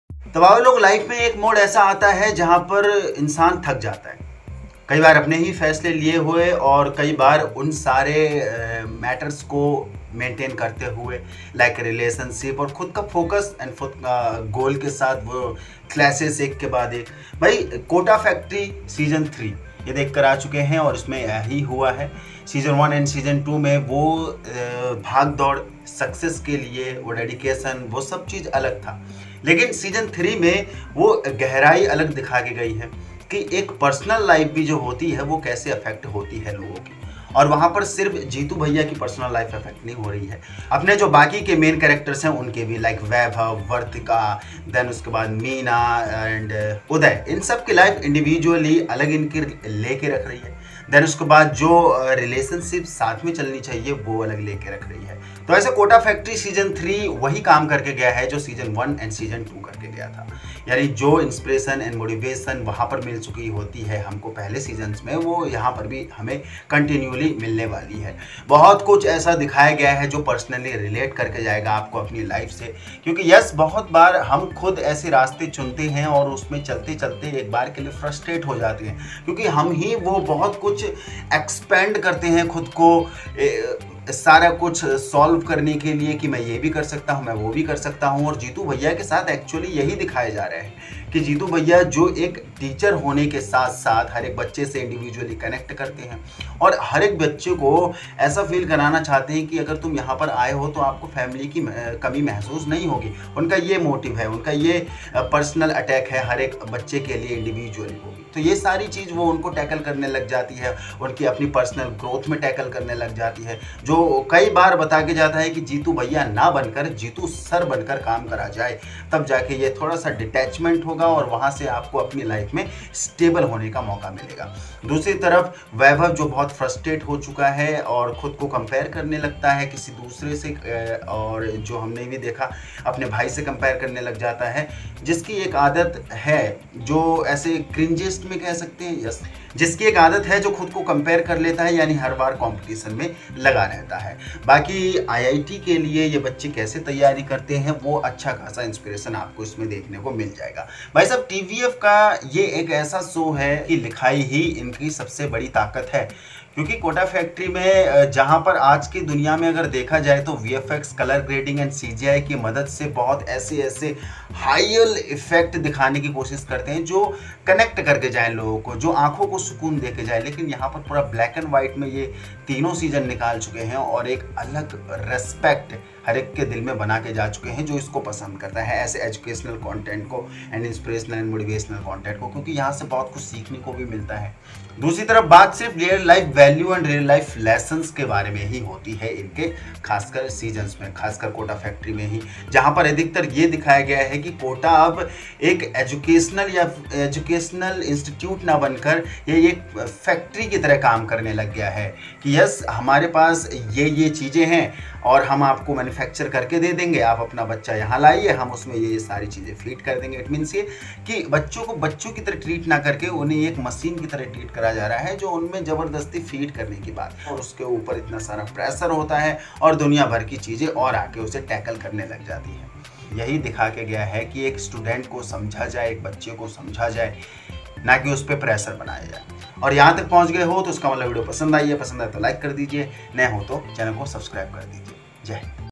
🔥Kota Factory Season 3 Review: Sach Me Life ki Ma ki Ankh Ho Rakhi Hai😟
Welcome back, viewers! In this video, we dive deep into Kota FACTory Season 3, the latest installment of the popular web series. Join us as we review each episode, discuss character developments, and analyze the plot twists that have left fans talking!

📚 What You'll Find in This Review:
Episode-by-Episode Breakdown: Detailed analysis of each episode.
Character Arcs: How Vaibhav, Jeetu Bhaiya, and others have evolved.
Themes and Messages: The core themes of Season 3 and their impact.
Favorite Moments: Our top picks from this season.
Viewer Reactions: What fans are saying about Kota FACTory Season 3.

Your support helps us bring you more content like this.